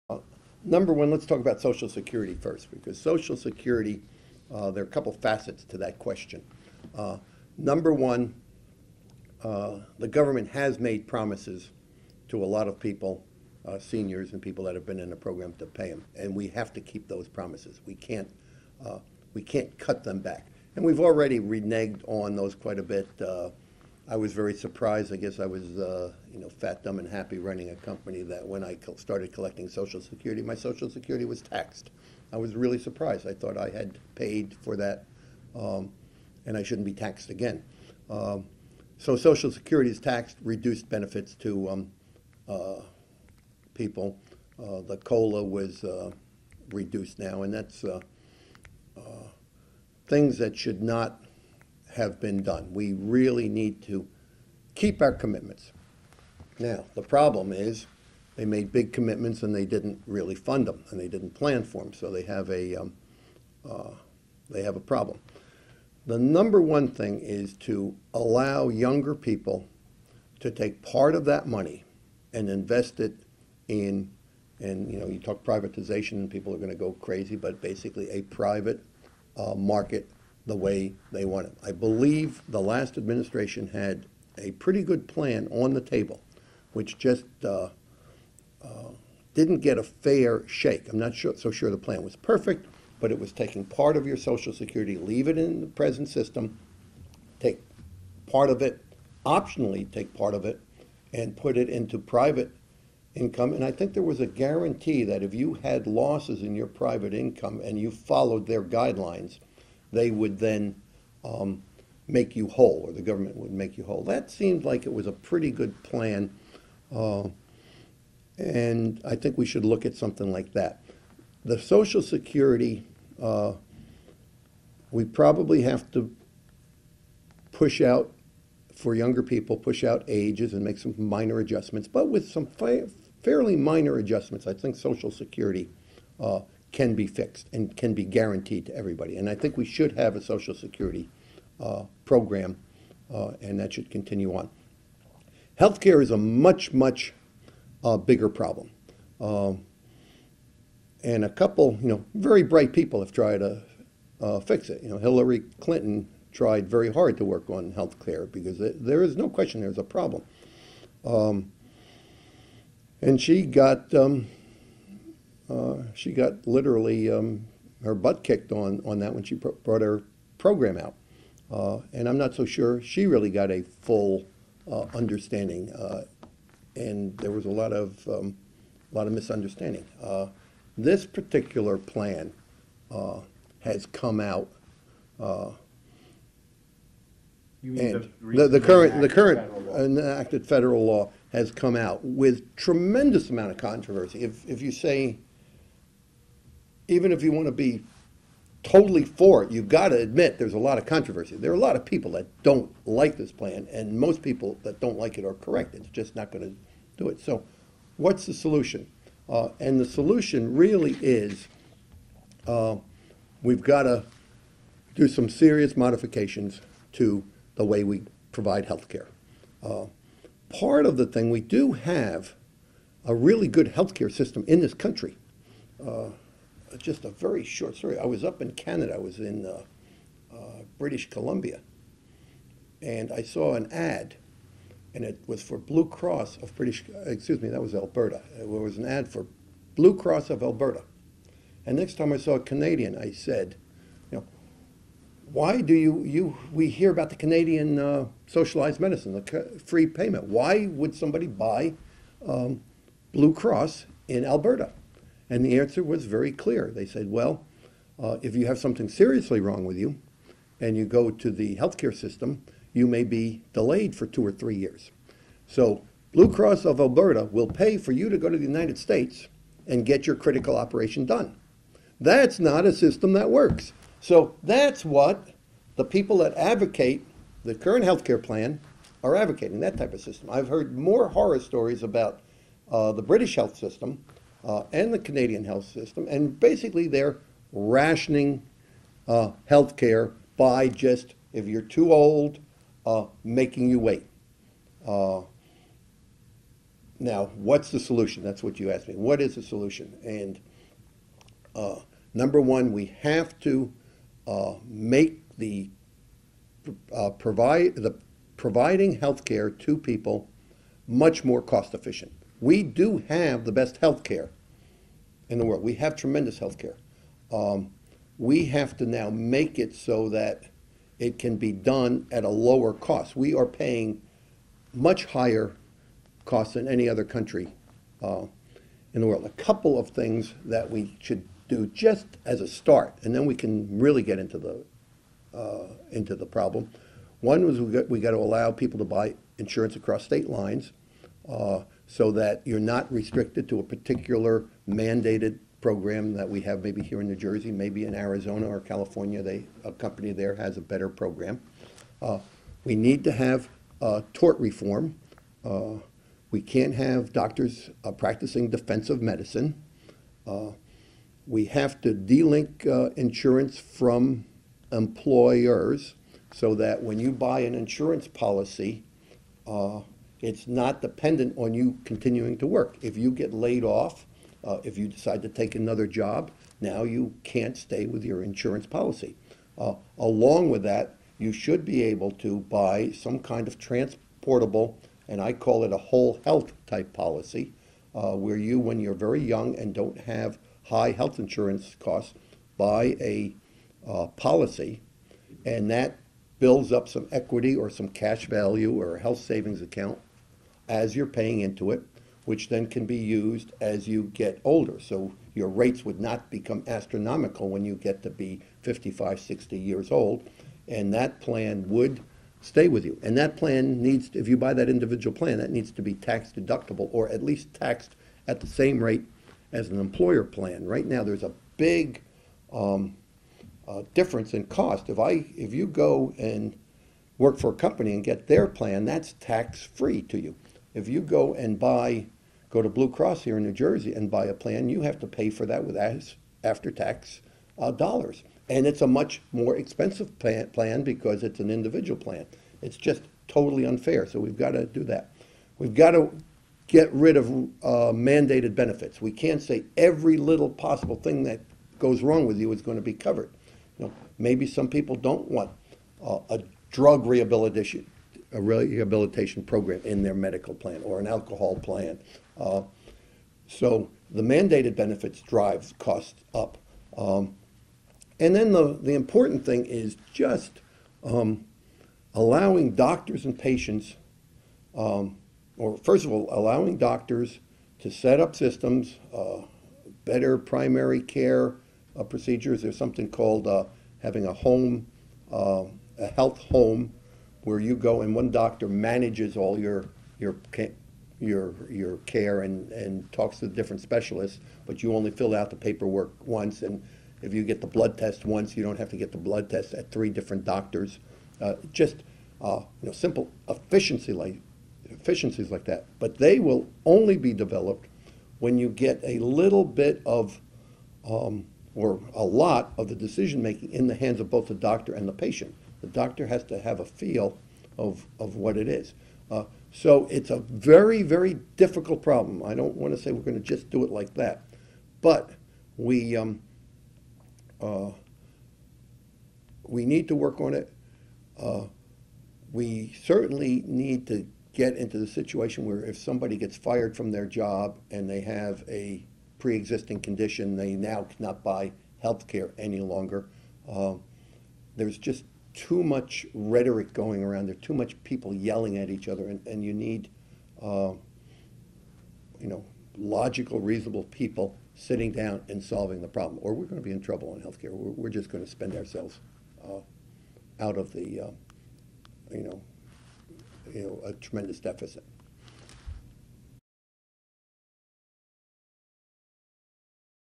Roland Straten on Social Security-Medicare Deficits
| Roland Straten, the Republican challenger in New Jersey's 8th Congressional District, discusses what changes to Medicare and Social Security he'd support and oppose.
This is part of a series produced for the 2010 election by The Record